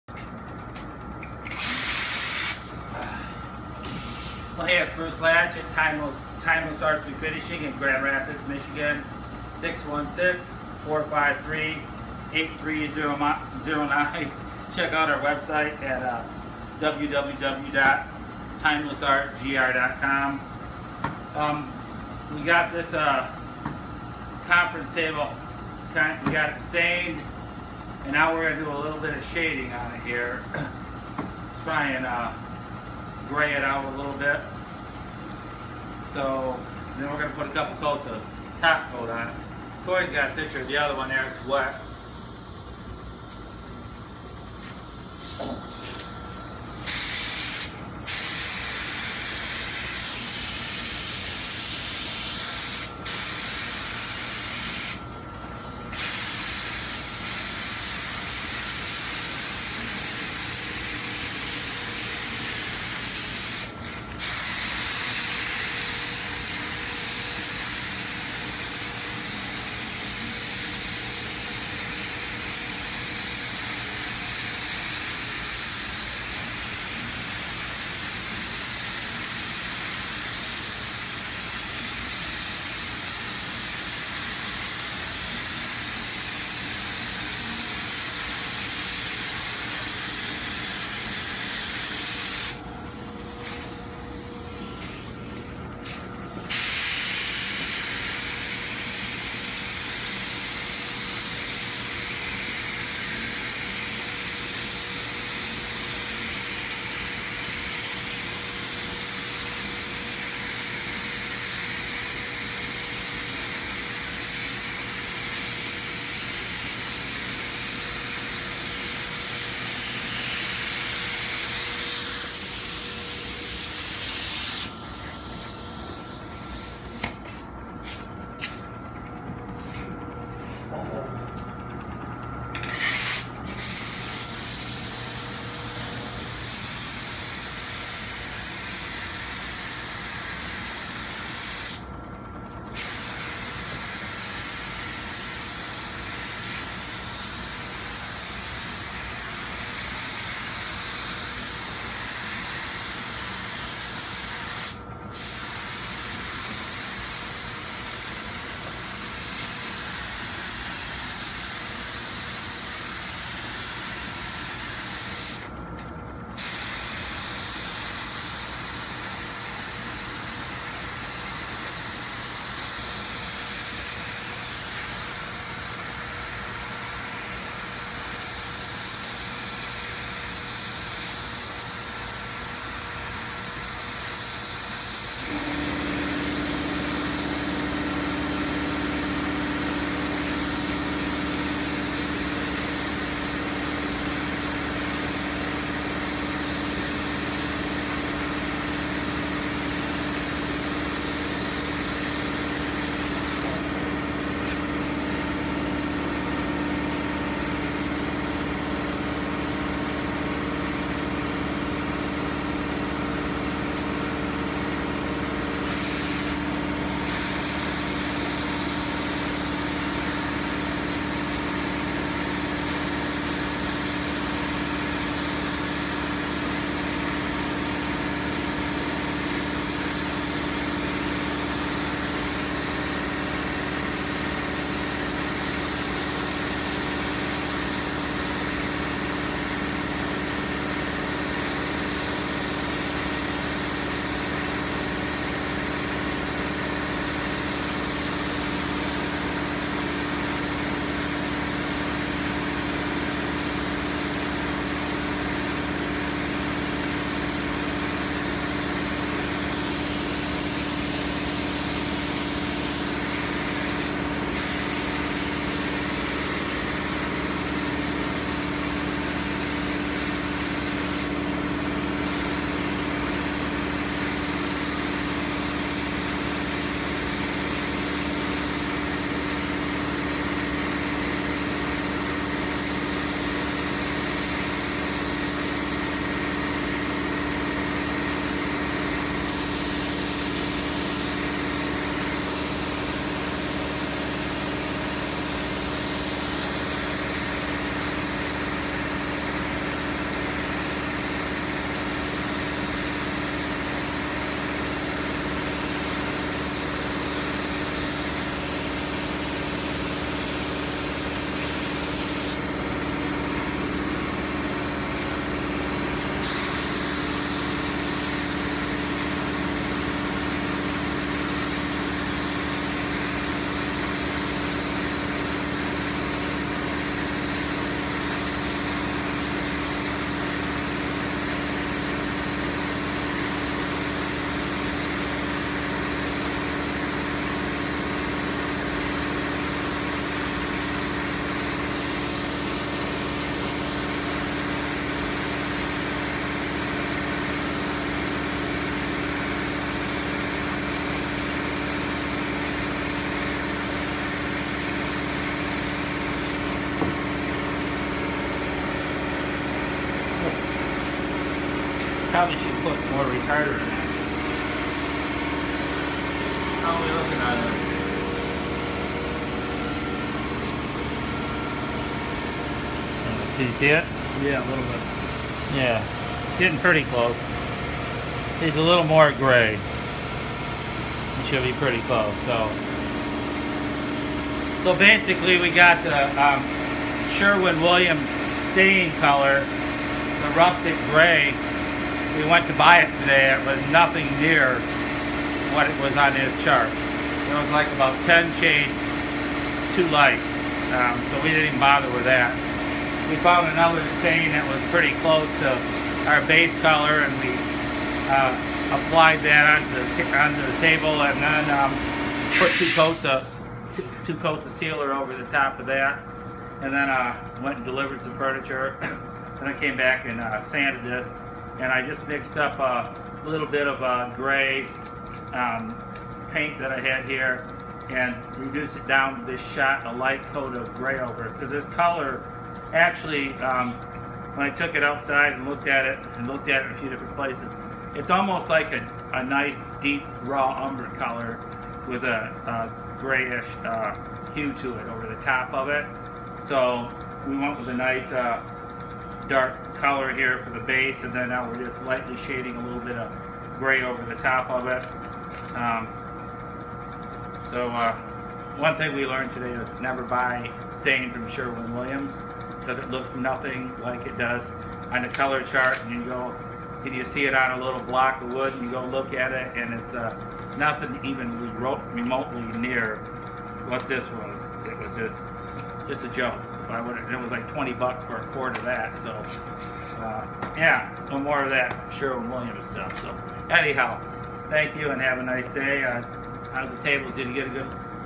refinishing conference table,s grandville mi
timeless arts refinishing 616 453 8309 show,s how we refinish large conference tables ,after stripping and sanding the table ,it is time to stained and sealed it and then shaded with a light grey color